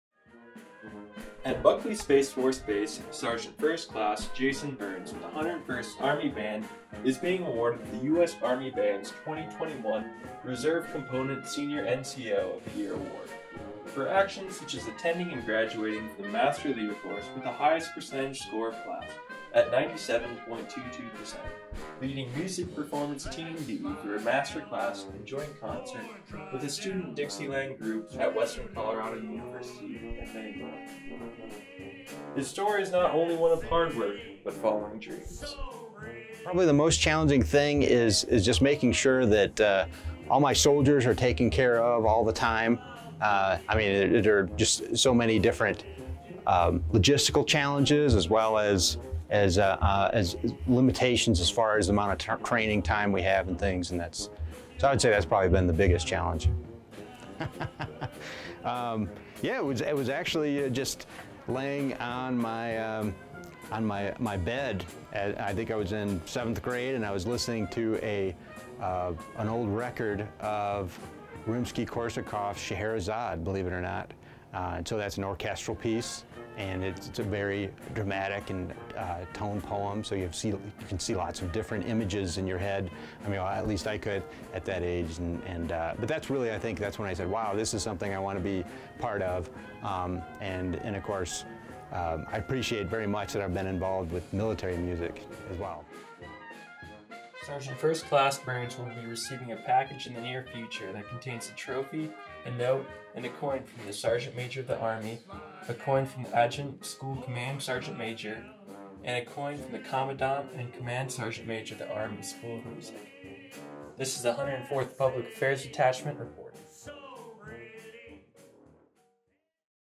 Army Band NCO of the Year 2021
U.S. Army Sgt. 1st Class Jason Byrnes receives the Army Band Reserve Component NCO of the year award for 2021, this is a profile of his story.